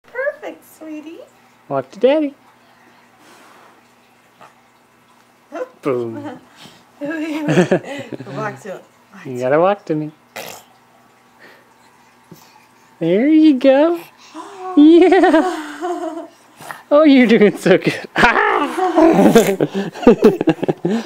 Coral Walking
Coral is actually walking now!